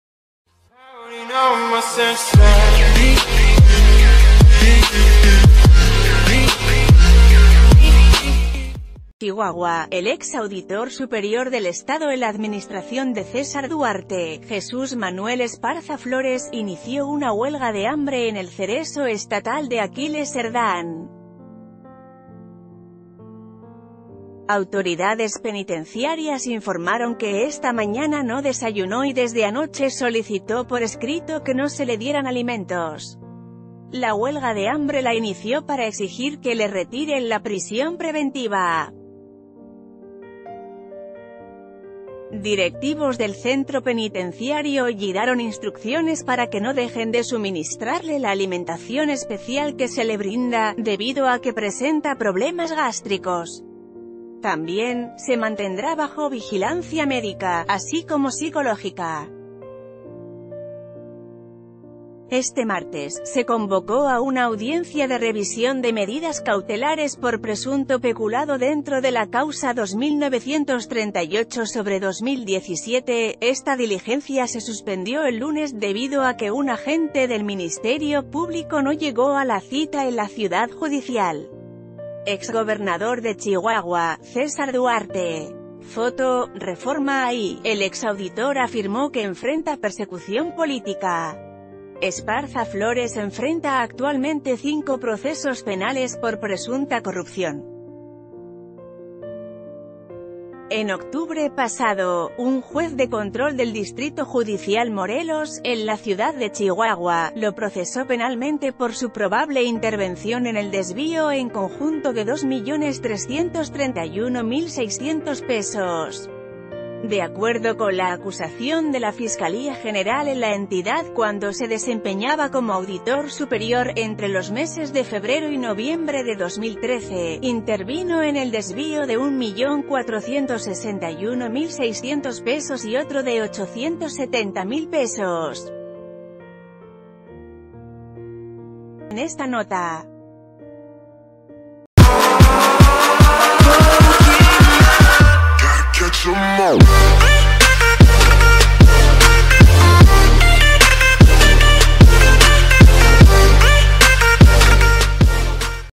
Ligado a César Duarte inicia huelga de hambre en el Cereso
Chihuahua.- El ex Auditor Superior del Estado en la Administración de César Duarte, Jesús Manuel Esparza Flores, inició una huelga de hambre en el Cereso Estatal de Aquiles Serdán. Autoridades penitenciarias informaron que esta mañana no desayunó y desde anoche solicitó por escrito que no se le dier...